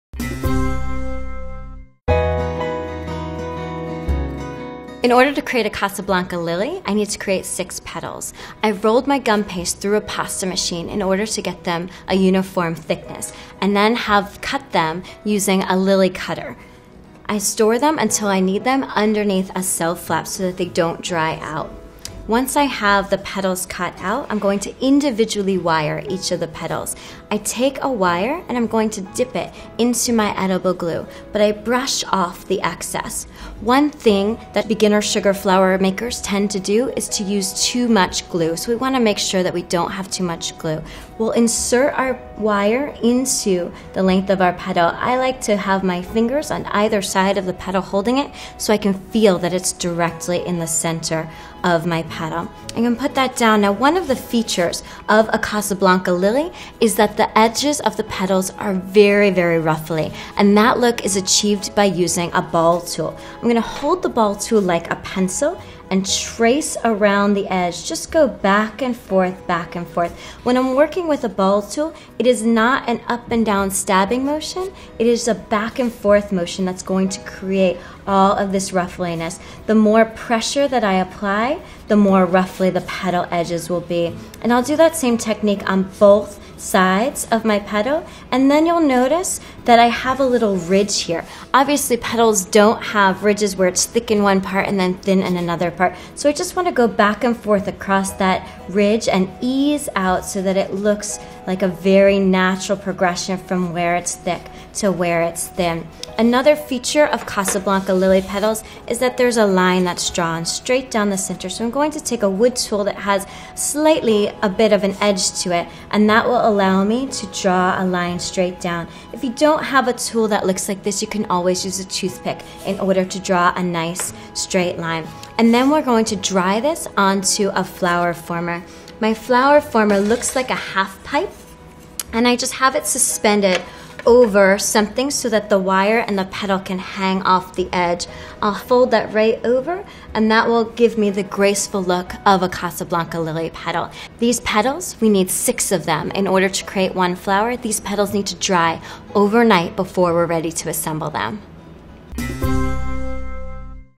How to Make Casablanca Lily Petals | Sugar Flowers
In order to create a Casablanca lily, I need to create six petals. I've rolled my gum paste through a pasta machine in order to get them a uniform thickness, and then have cut them using a lily cutter. I store them until I need them underneath a cell flap so that they don't dry out.

Once I have the petals cut out, I'm going to individually wire each of the petals. I take a wire, and I'm going to dip it into my edible glue, but I brush off the excess. One thing that beginner sugar flower makers tend to do is to use too much glue. So we want to make sure that we don't have too much glue. We'll insert our wire into the length of our petal. I like to have my fingers on either side of the petal holding it so I can feel that it's directly in the center of my petal. I'm going to put that down now.

One of the features of a Casablanca lily is that the edges of the petals are very, very ruffly. And that look is achieved by using a ball tool. I'm going to hold the ball tool like a pencil and trace around the edge. Just go back and forth, back and forth. When I'm working with a ball tool, it is not an up and down stabbing motion. It is a back and forth motion that's going to create all of this ruffliness. The more pressure that I apply, the more ruffly the petal edges will be. And I'll do that same technique on both sides of my petal.

And then you'll notice that I have a little ridge here. Obviously petals don't have ridges, where it's thick in one part and then thin in another part. So I just want to go back and forth across that ridge and ease out so that it looks like a very natural progression from where it's thick to where it's thin.

Another feature of Casablanca lily petals is that there's a line that's drawn straight down the center. So I'm going to take a wooden tool that has slightly a bit of an edge to it, and that will allow me to draw a line straight down. If you don't have a tool that looks like this, you can always use a toothpick in order to draw a nice straight line.

And then we're going to dry this onto a flower former. My flower former looks like a half-pipe, and I just have it suspended over something so that the wire and the petal can hang off the edge. I'll fold that right over, and that will give me the graceful look of a Casablanca lily petal.

These petals, we need six of them in order to create one flower. These petals need to dry overnight before we're ready to assemble them.